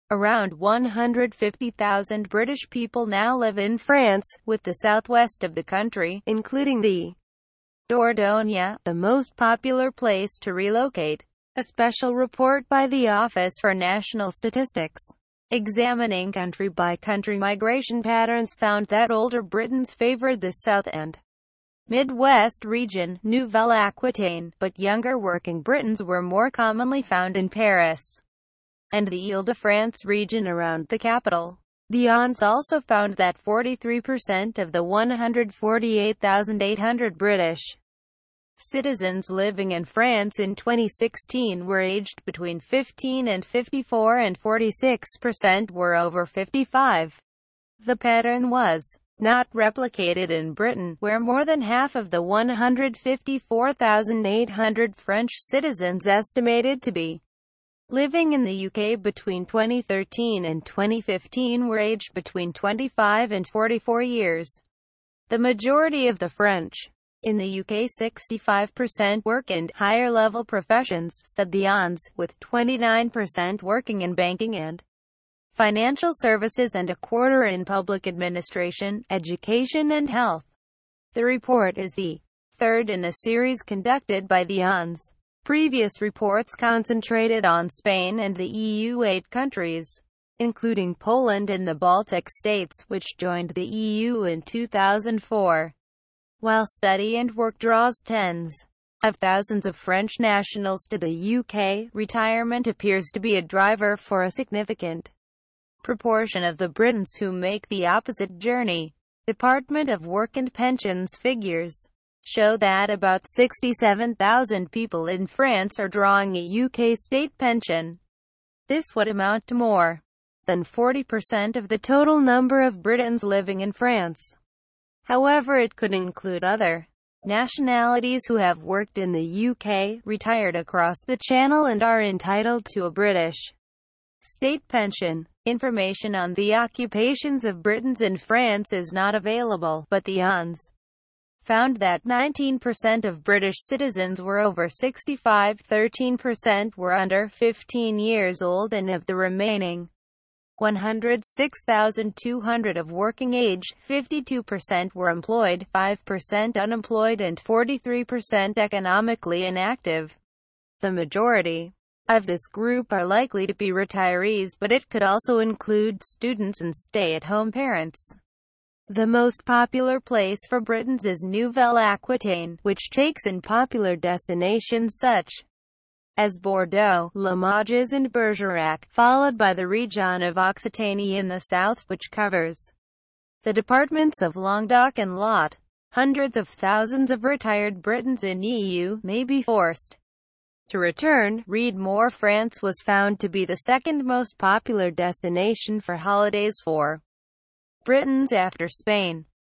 British Pensioners The Most Likely To Start A New Life In France
--  Around 150 000 British people now live in France  with the south west of the country  including the Dordogne  the most popular place to relocate.  A special report by the Office for National Statistics examining country by country migration patterns found that older Britons favoured the south and mid west region  Nouvelle Aquitaine  but younger working Britons were more commonly found in Paris and the  le de France region around the capital.  The ONS also found that 43  of the 148 800 British citizens living in France in 2016 were aged between 15 and 54 and 46  were over 55.   The pattern was not replicated in Britain  where more than half of the 154 800 French citizens estimated to be living in the UK between 2013 and 2015 were aged between 25 and 44 years.   The majority of the French in the UK   65    work in  higher level professions  said the ONS  with 29  working in banking and financial services and a quarter in public administration  education and health.   The report is the third in a series conducted by the ONS.  Previous reports concentrated on Spain and the EU8 countries including Poland and the Baltic states  which joined the EU in 2004.   While study and work draws tens of thousands of French nationals to the UK  retirement appears to be a driver for a significant proportion of the Britons who make the opposite journey.   Department of Work and Pensions figures show that about 67 000 people in France are drawing a UK state pension.  This would amount to more than 40  of the total number of Britons living in France.  However it could include other nationalities who have worked in the UK  retired across the channel and are entitled to a British state pension.  Information on the occupations of Britons in France is not available  but the ONS found that 19  of British citizens were over 65  13  were under 15 years old and of the remaining 106 200 of working age  52  were employed  5  unemployed and 43  economically inactive.  The majority of this group are likely to be retirees but it could also include students and stay at home parents.  The most popular place for Britons is Nouvelle Aquitaine  which takes in popular destinations such as Bordeaux  Limoges and Bergerac  followed by the region of Occitanie in the south  which covers the departments of Languedoc and Lot.       Hundreds of thousands of retired Britons in EU  may be forced to return            Read more       France was found to be the second most popular destination for holidays for Britons after Spain.   The ONS said that  although the numbers of UK citizens living in France and French nationals resident in the UK are  roughly similar    of the two it seems France is the more popular  retirement  destination .  The number of French people the ONS calculated to be living in the UK is much lower than the 300 000 widely reported to be in the country.   With no requirement for any EU citizen to register in the UK  the ONS looked to a number of sources including the Census  Eurostat  the Labour Force Survey and the Annual Population Survey to arrive at its estimates--